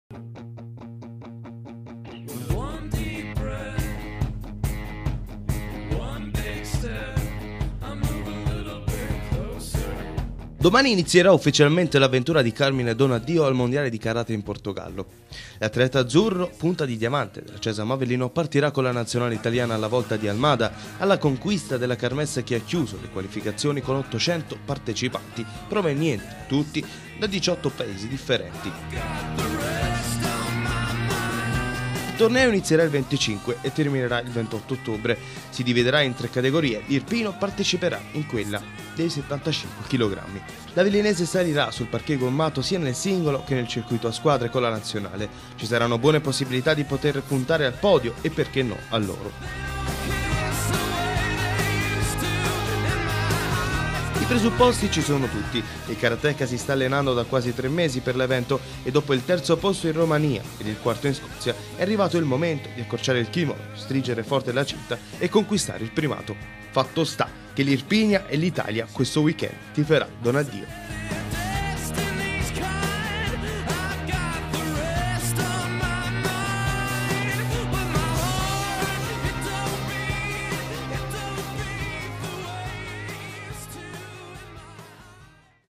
Karate, l'Italia tifa Donadio
L'irpino partirà domani alla volta di Almada dove ritroverà i compagni della nazionale. La competizione inizierà il 25 e terminerà il 28, l'atleta gareggierà sia nel singolo che nel circuito a squadre